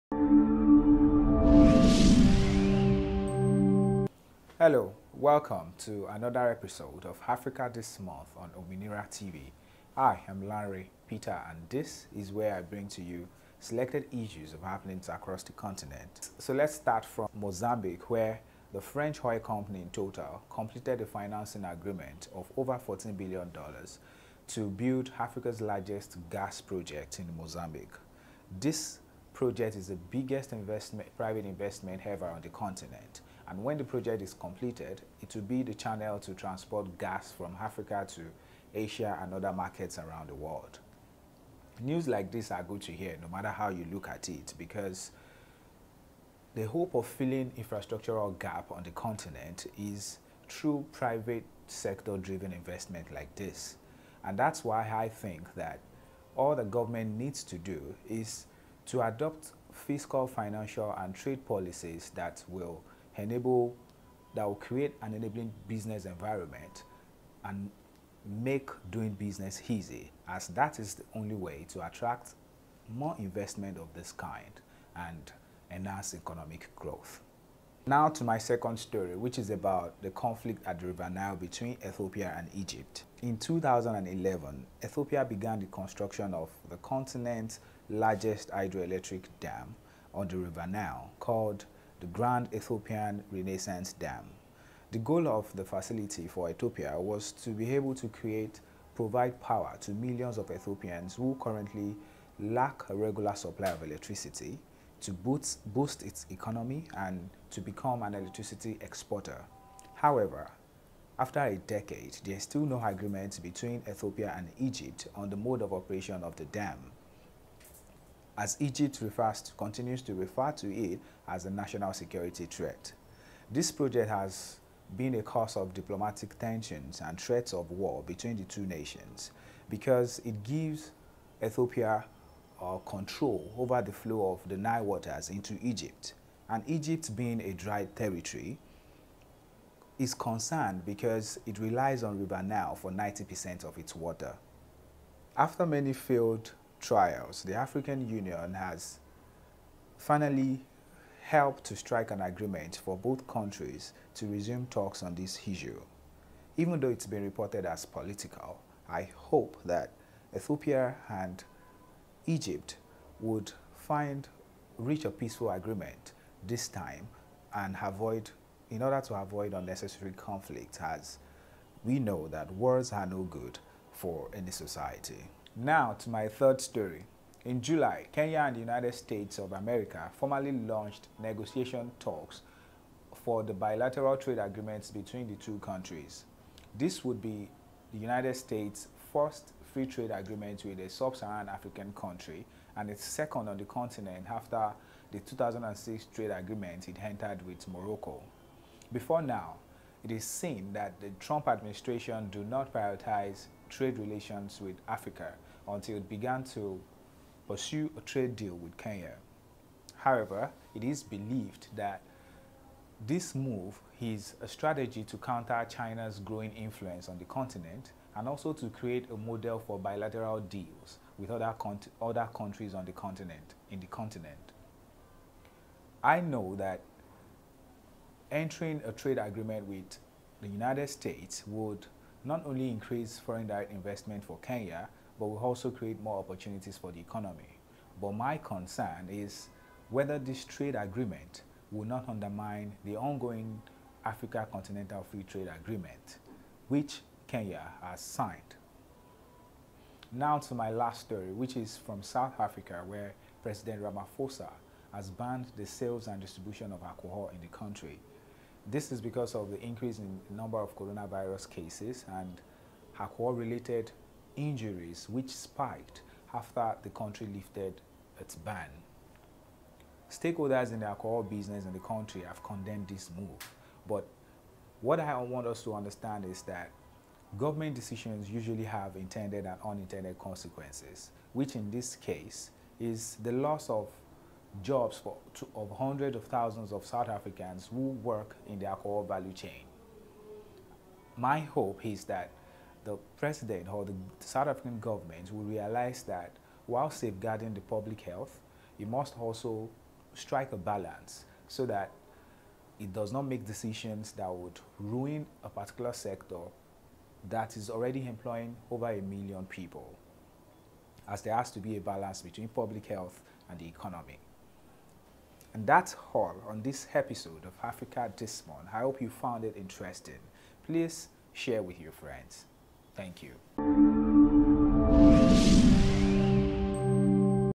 Africa This Month with Lanre Peter | July 2020.
Every month Lanre brings you a breakdown of selected issues happening around the continent.

Here he talks about Africa's biggest private investment launching a gas project in Mozambique, the US-Kenya trade agreement, the conflict between Ethiopia and Egypt over the Grand Ethiopian Dam on the river Nile and more.